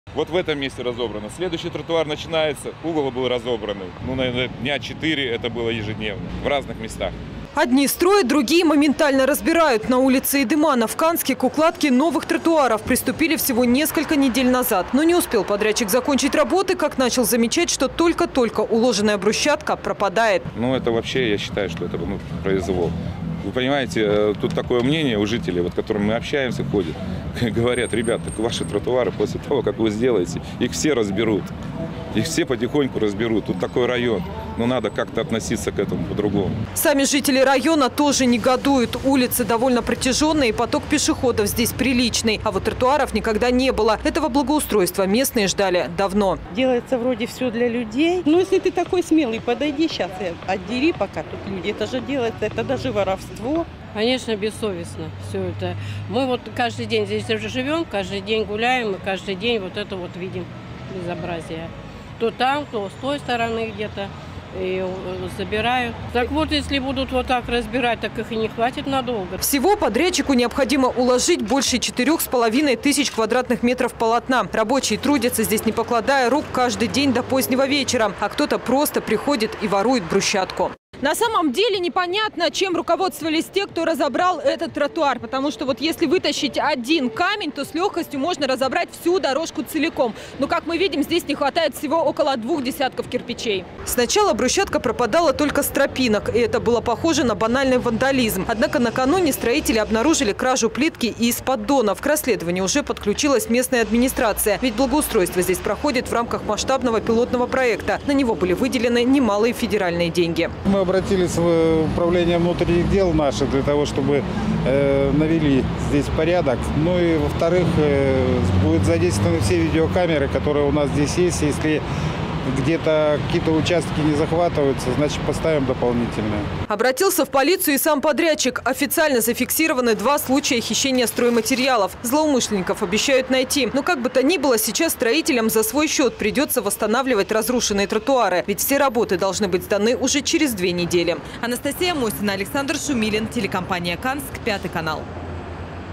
УКРАЛИ БРУСЧАТКУ
На Эйдемана, где идет благоустройство улицы, неизвестные в ночное время вынимают новую брусчатку из уже уложенных тропинок.